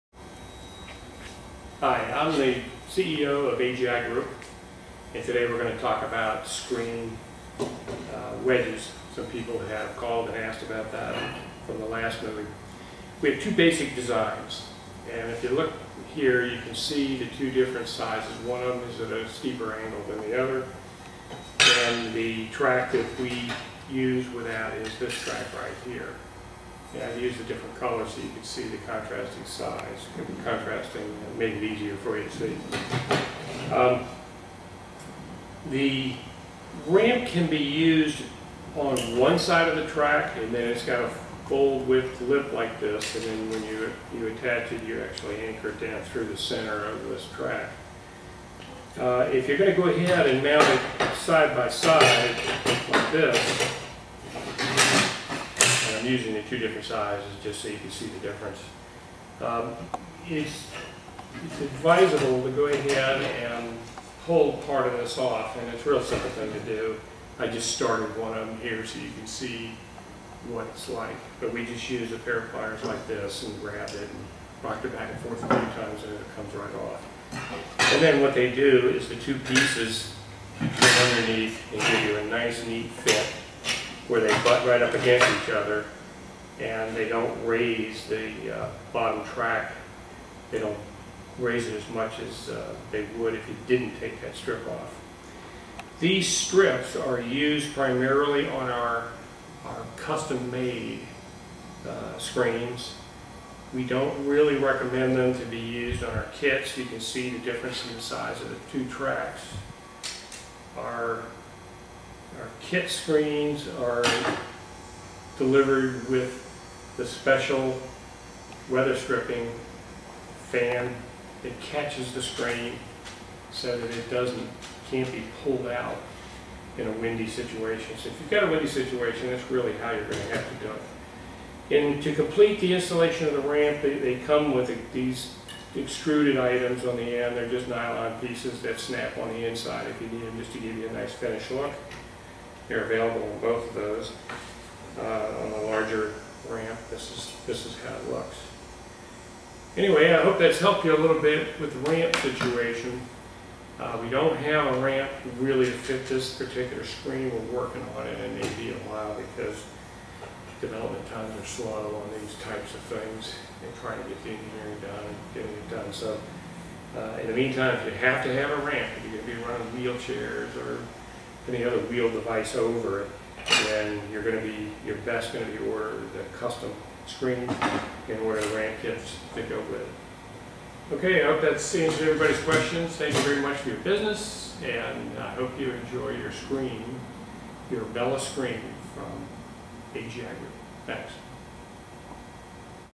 Bella Retractable Screen Ramp
A detailed review of how the accessibility Ramp works with Bella Retractable Screens.